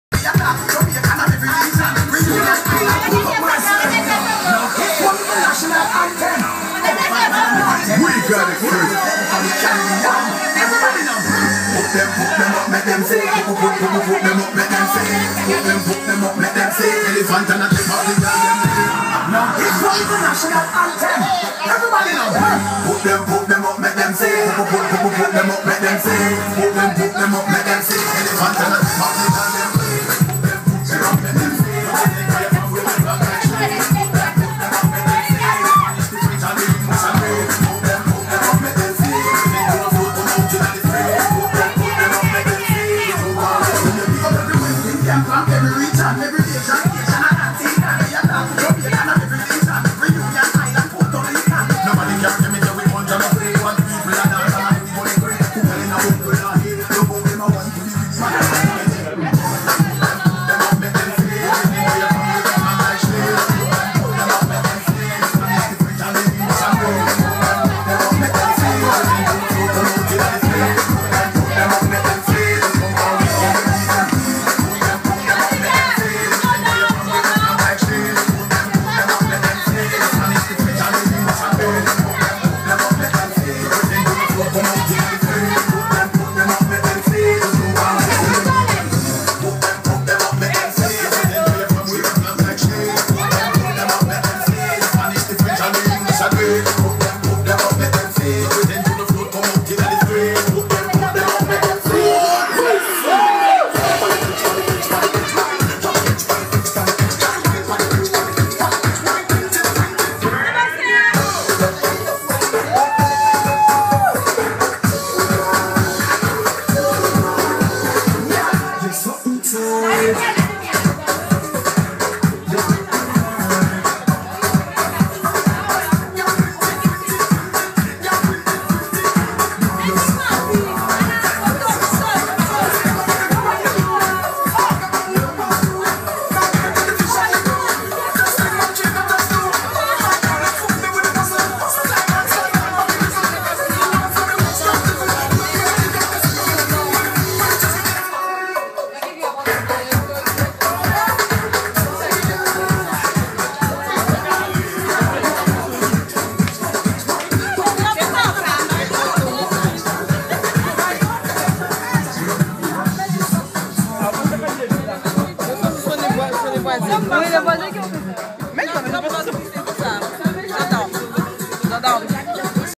Fete mixte des pères et anniversaire de Jessica
A un endoit dans le sud de l'ile